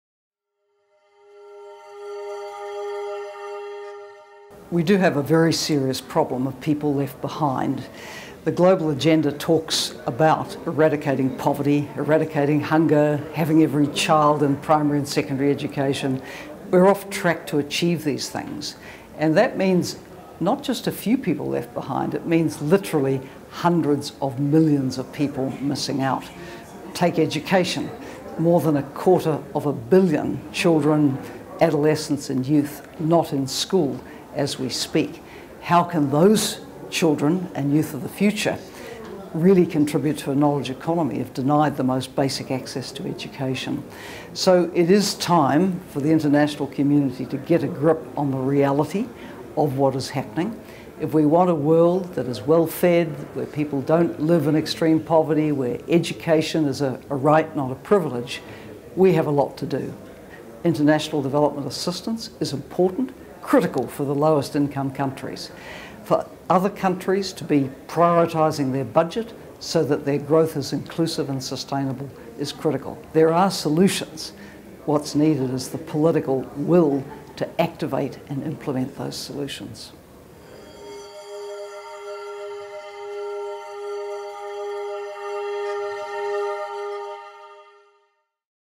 Should we work harder to include the left behind? Helen Clark, former Prime Minister, New Zealand
Helen Clark, former Prime Minister, New Zealand